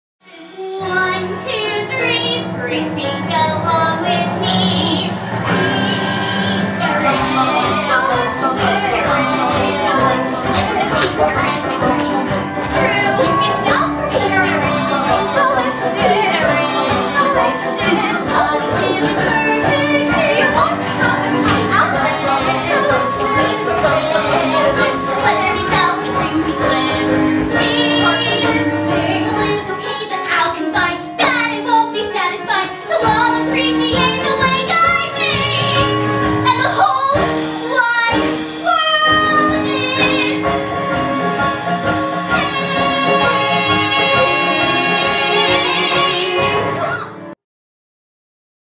"Pinkalicious" Presented by The St. Louis Family Theatre Series
In this Vital Theatre Company production, Pinkalicious can’t stop eating pink cupcakes despite warnings from her parents. Her pink indulgence lands her at the doctor’s office with Pinkititis, an affliction that turns her pink from head to toes – a dream come true for this pink loving enthusiast. But when her hue goes too far, only Pinkalicious can figure out a way to get out of this predicament.